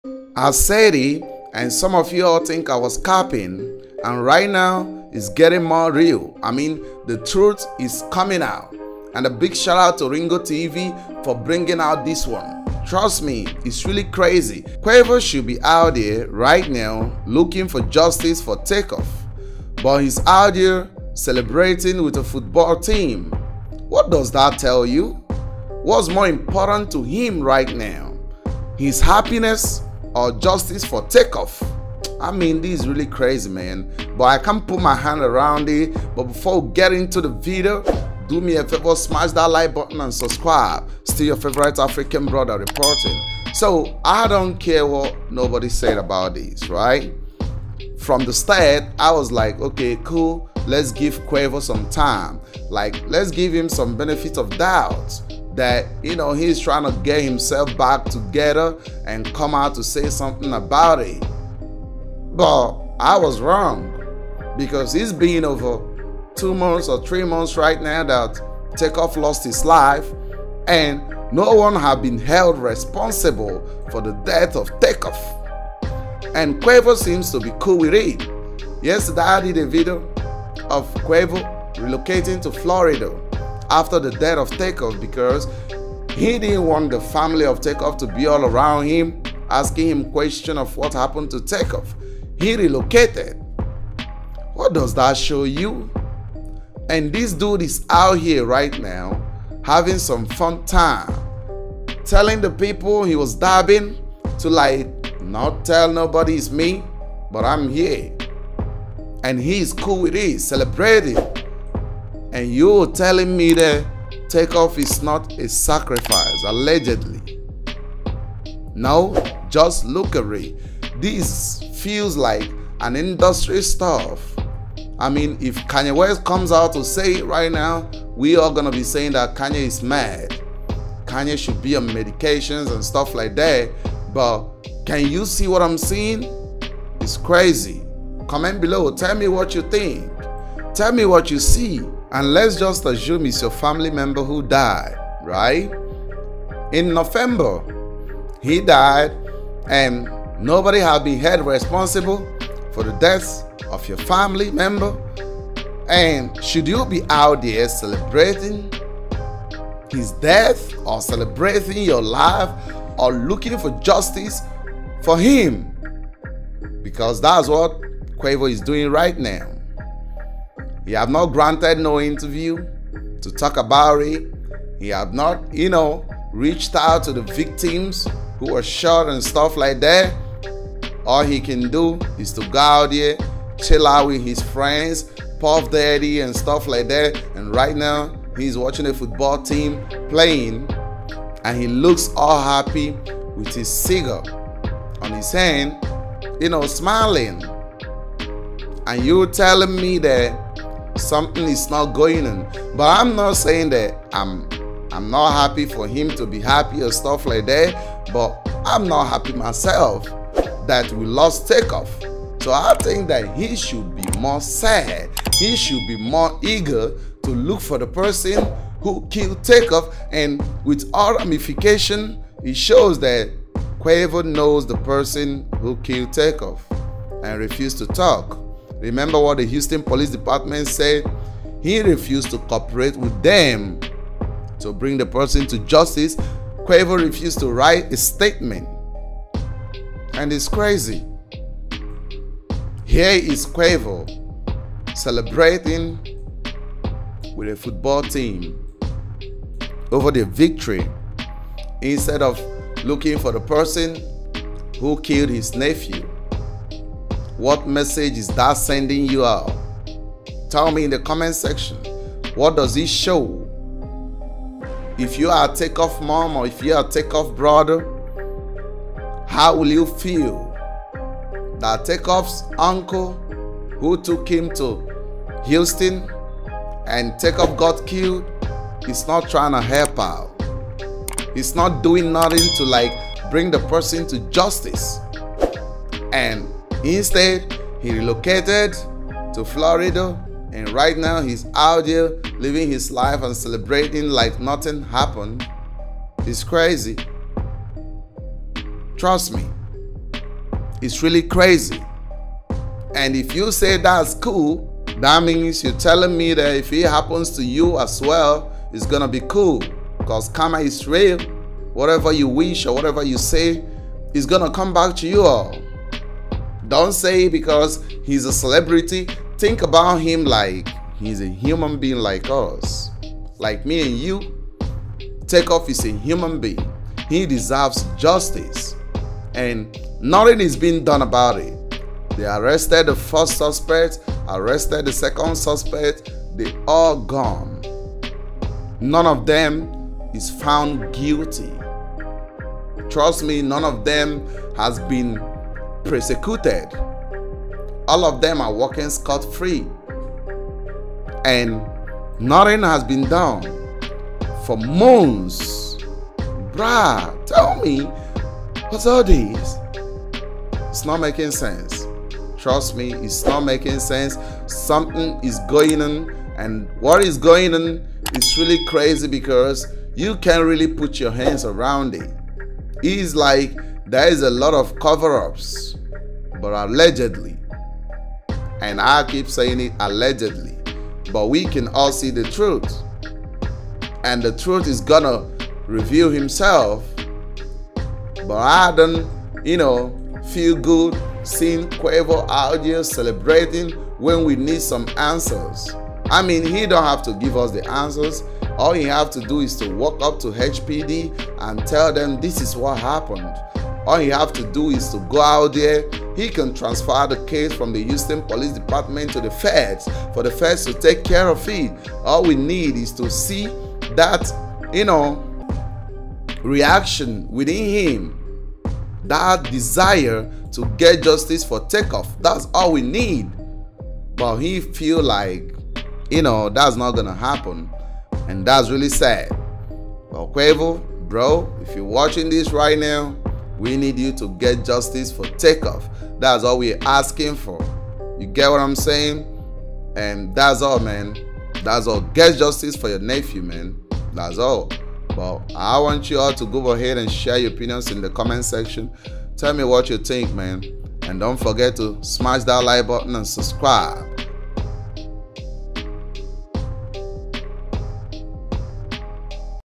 QUAVO Celebrating After TakeOff’s D**TH
Quavo out there celebrating with a football team than trying to get Justice for his nephew TakeOff.

NOTE: EVERYTHING SAID IN THIS VIDEO IS ALLEGEDLY